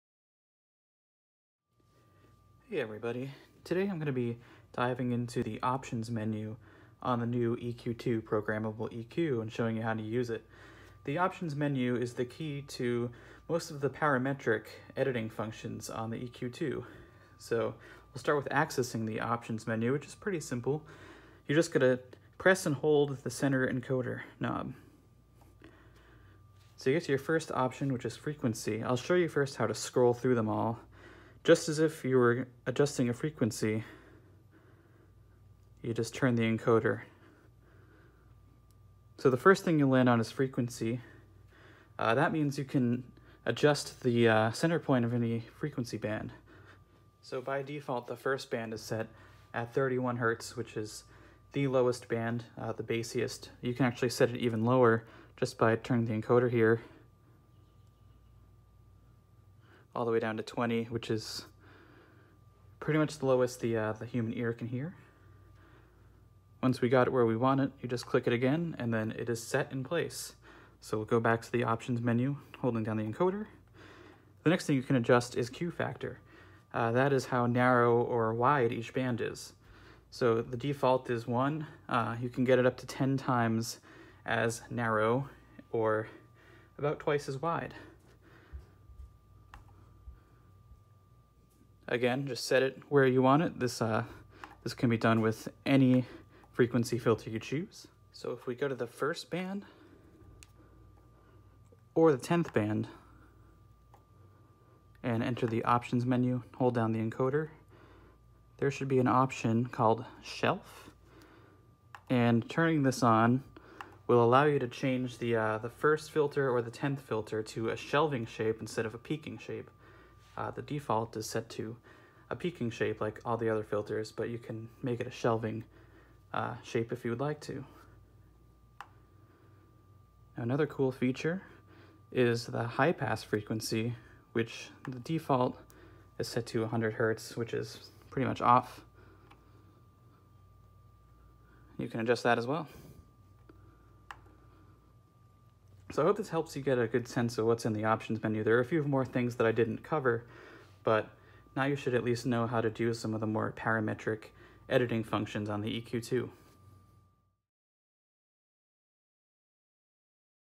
Source Audio EQ2 - Navigating the Options Menu Quick Tutorial
A quick rundown of how to navigate the EQ2 Programmable EQ's Options Menu to get to those parametric editing functions.